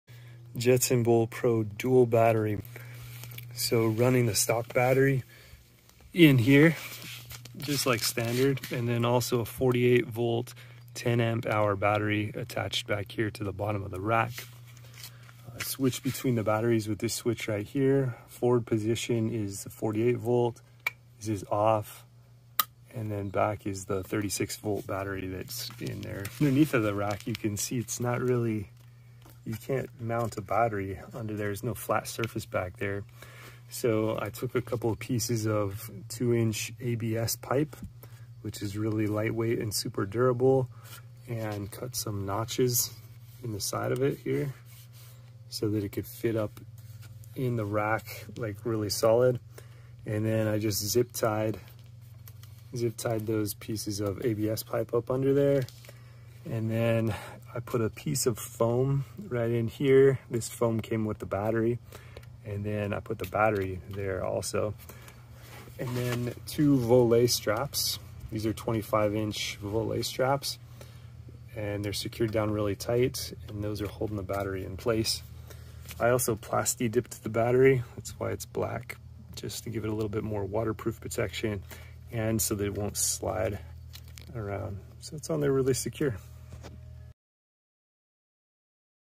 Jetson Bolt Pro Dual Battery Mod
Add a second battery while keeping the center of gravity low and keeping space available on the cargo rack.

Cargo Rack: Lumintrail Bike Cargo Rack, 55 LBS Weight Capacity

2nd battery: 48v 10ah eBay seller power_batteryb